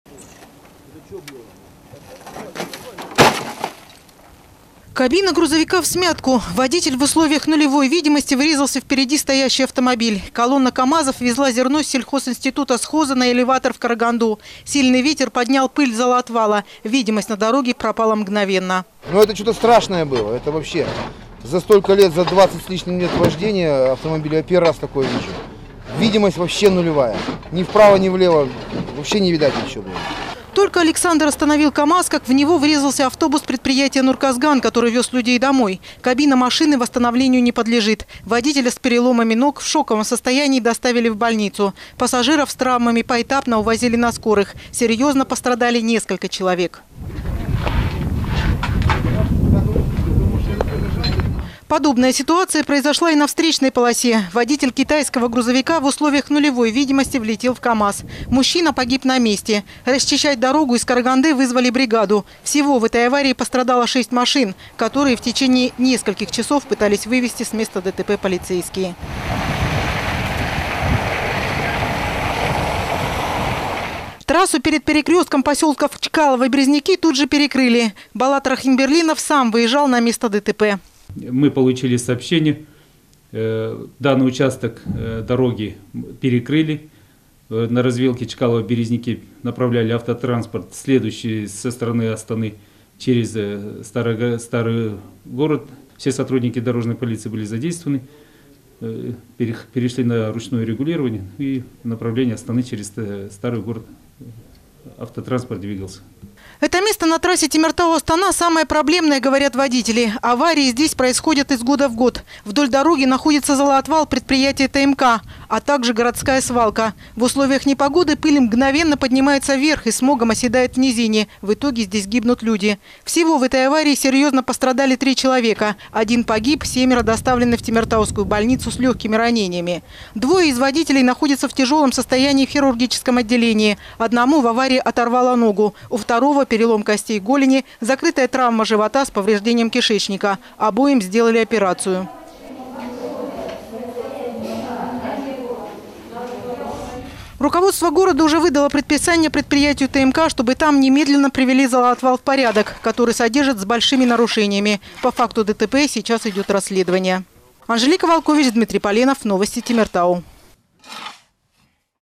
Из-за непогоды произошла крупная авария на трассе Темиртау-Астана
Перед перекрестком поселков Чкалова и Березняки столкнулись шесть автомобилей, в том числе грузовики и пассажирский автобус. Один человек погиб, трое получили серьезные травмы.